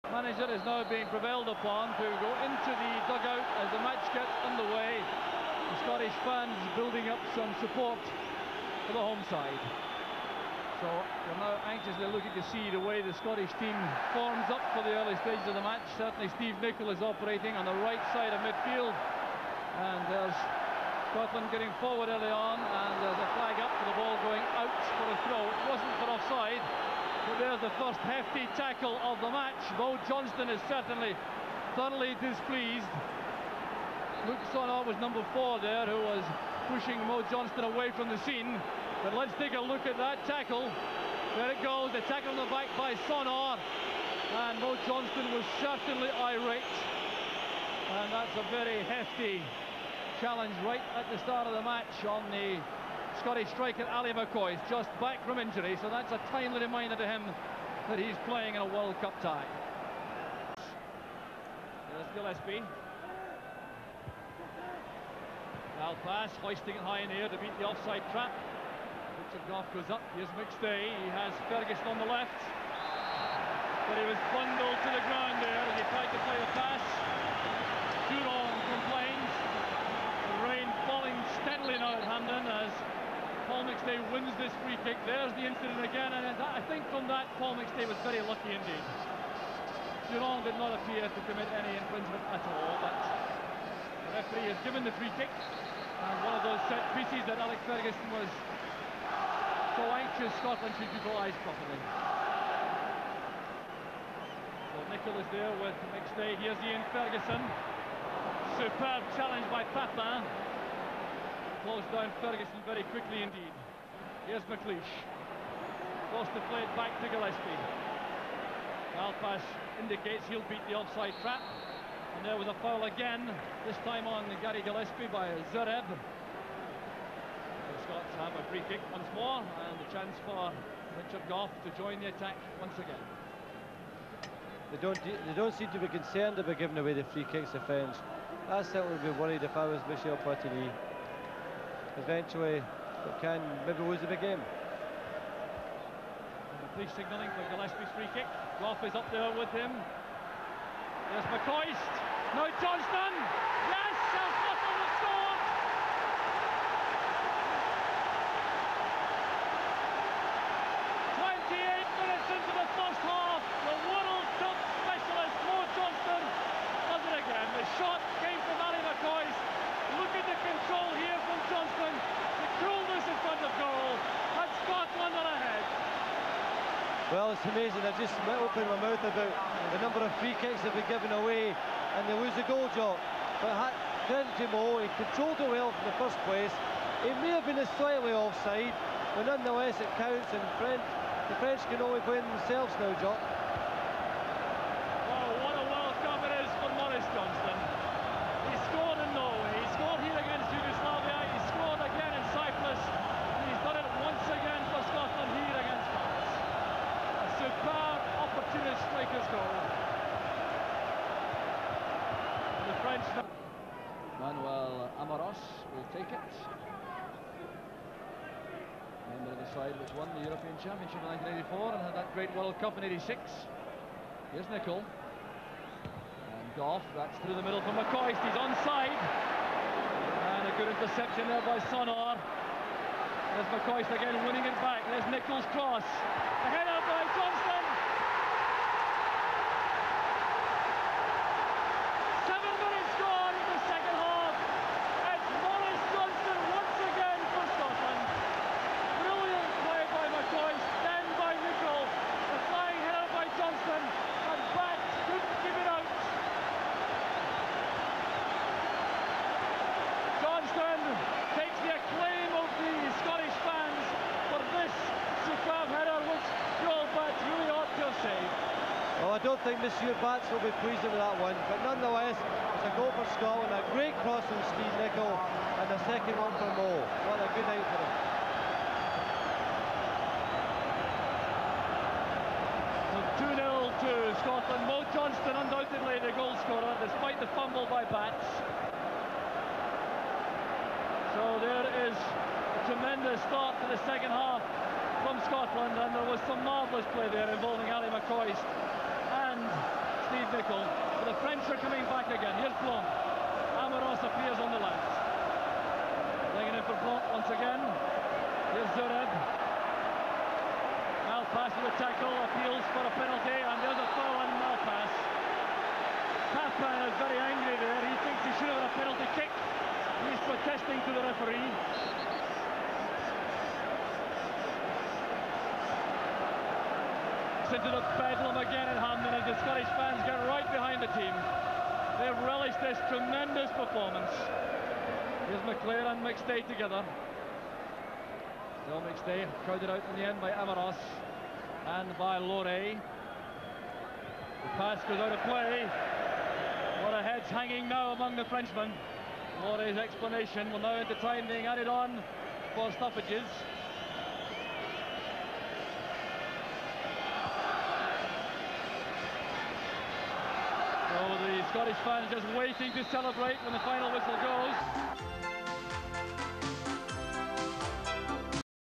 Scotland 2-0 France 1990 World Cup Qualifier
Match played at Hampden on March 8th, 1989. Full match available via a youtube private link. Also, for full match on DVD, go